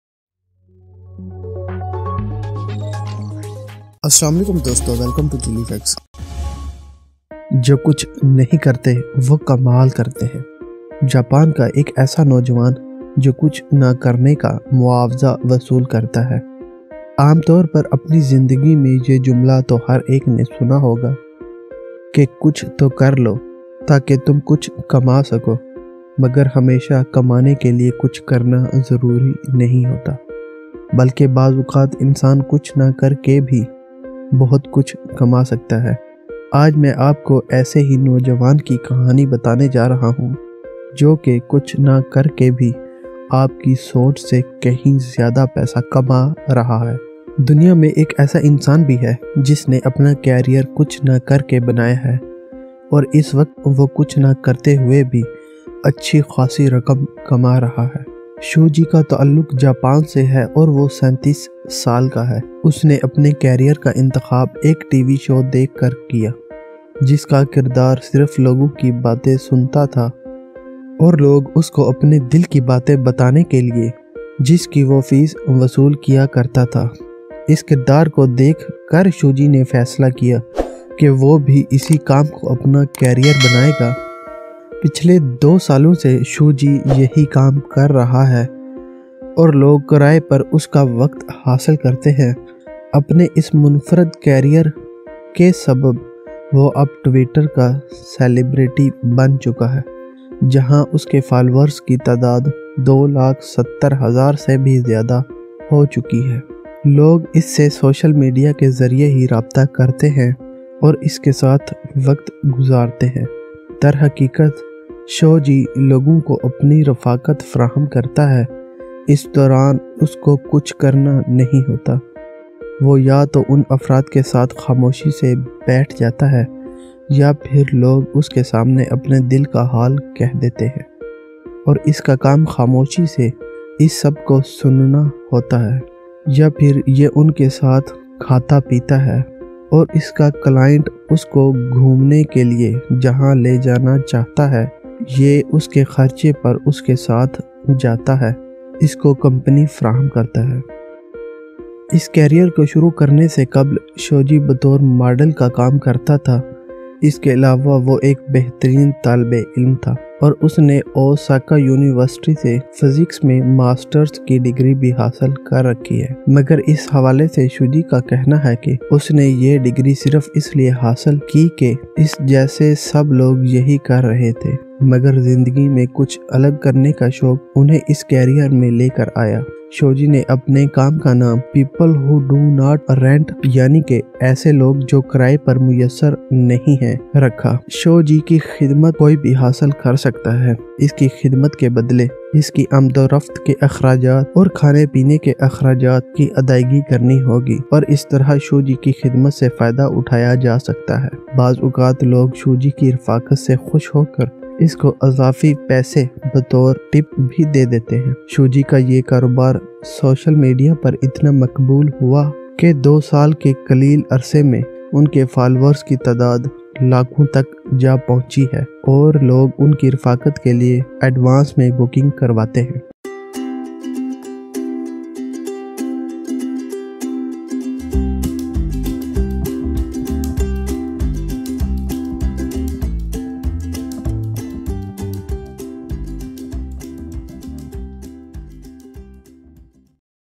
Success Story of Japanese men Shoji Morimoto Who Gets Paid to Do Nothing | chilli facts Hindi / Urdu
A 37-year-old Japanese man has built a successful career by renting himself out to do nothing. For some reason, he is inundated with requests from people who just want him by their side.